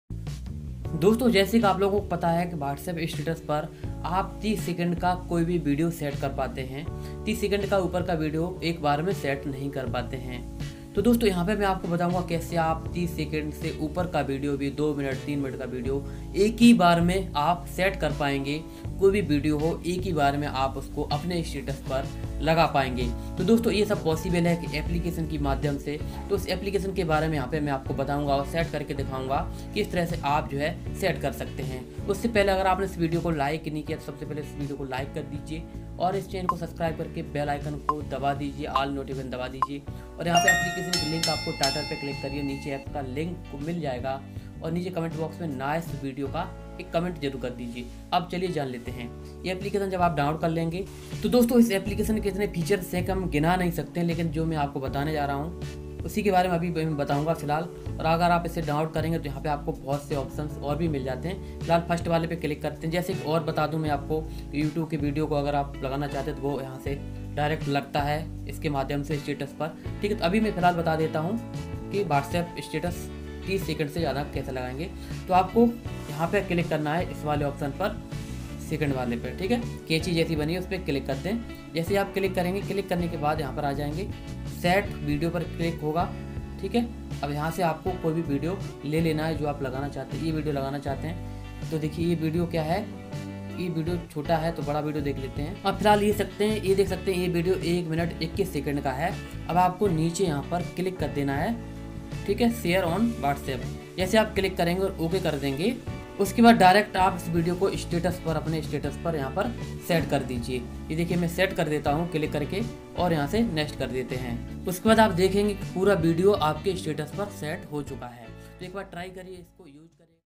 Whatsapp Status Par Ak Baar Me 30 Second SE bada Video kaise lagaye ?
Hello guys aajke Video Me aapko Bataya Gya hai kaise aap whatsapp status par 30 second se bada video apne status par laga skate hai..